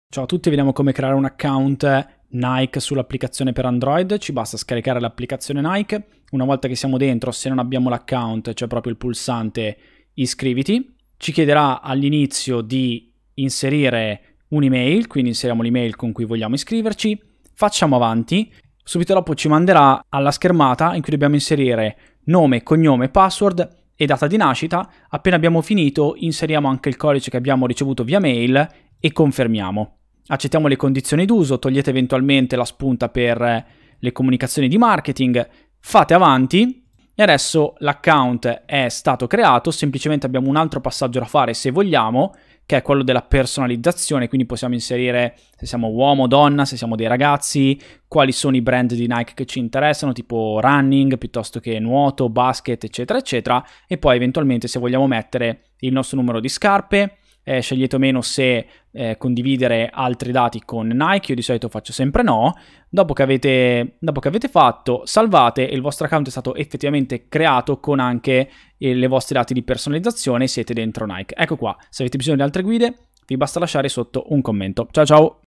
Come creare account Nike da Android
Vediamo come possiamo creare un account Nike da un qualsiasi dispositivo Android.